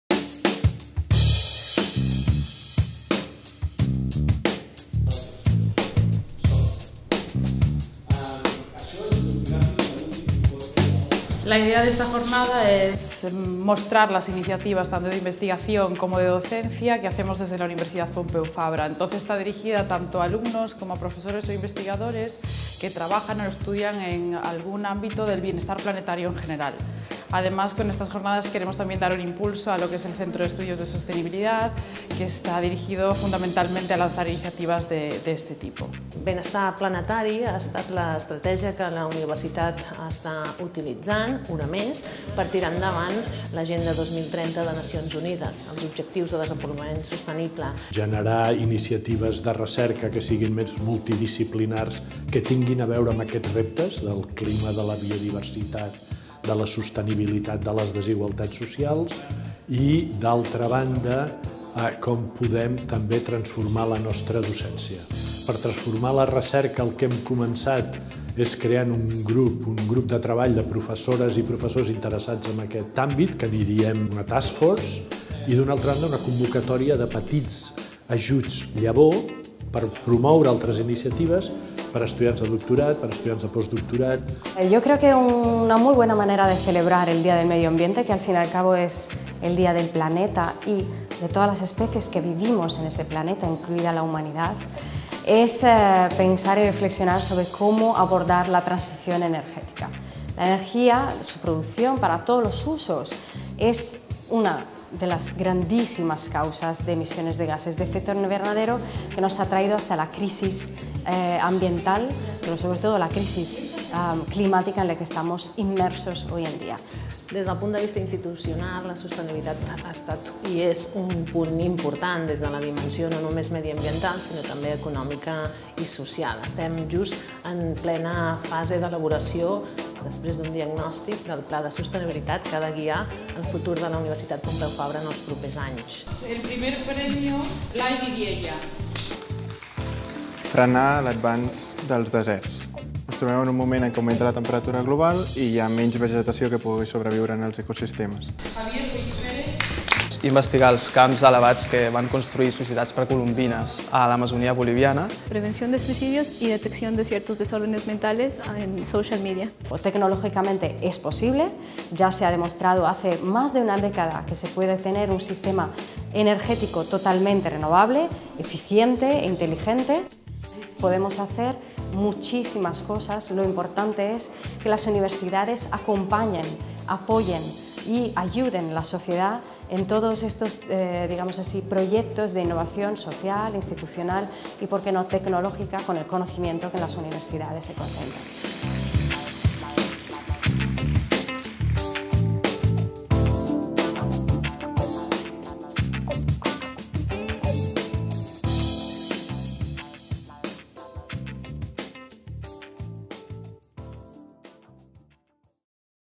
Jornada sobre Benestar Planetari
Organitzada pel Centre d'Estudis UPF sobre Sostenibilitat, va tenir lloc el 5 de juny,  a la sala Polivalent de l'edifici Mercè Rodoreda del campus de la Ciutadella, coincidint amb el Dia Mundial del Medi Ambient